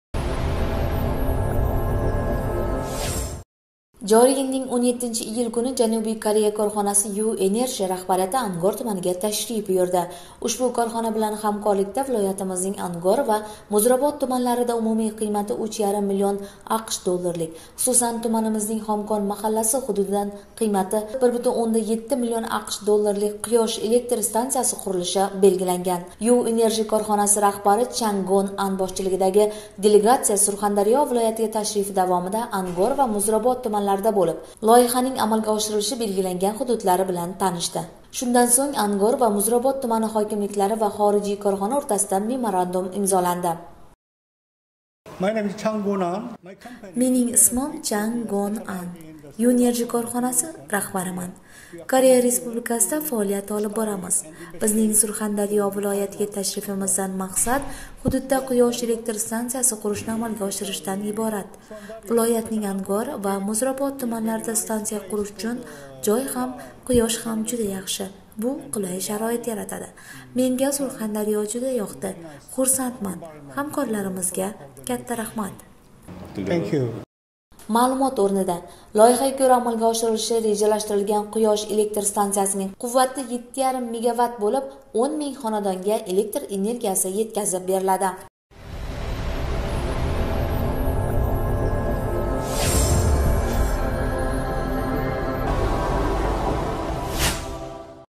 2022년 7월 17일 우즈베키스탄 수한다리오 주 앙고르 시 방문행사 방송
2022년 7월 17일 우즈베키스탄 수한다리오 주 앙고르 시 방문행사 방송
Janubiy Koreya korxonasi 'U Energy' Co LTD rahbariyati Angor tumaniga tashrif buyurdi